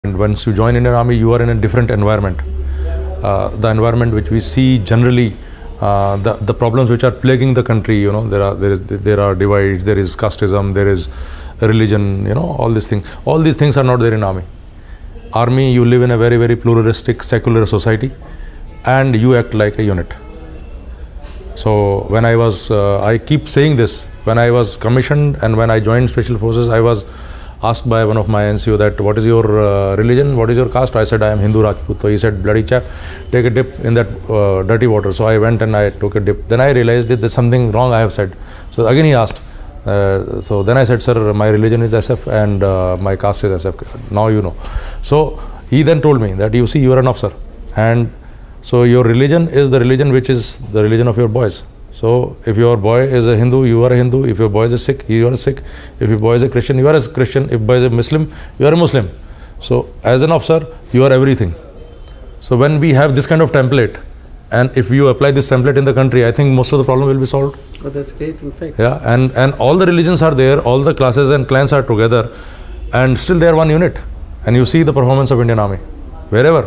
PARA SF - Brig. Saurabh Singh Sekhawat, GOLDEN WORDS , Must watch .
This is video , for all defence aspirants , some golden words by col Saurabh Shekhawat .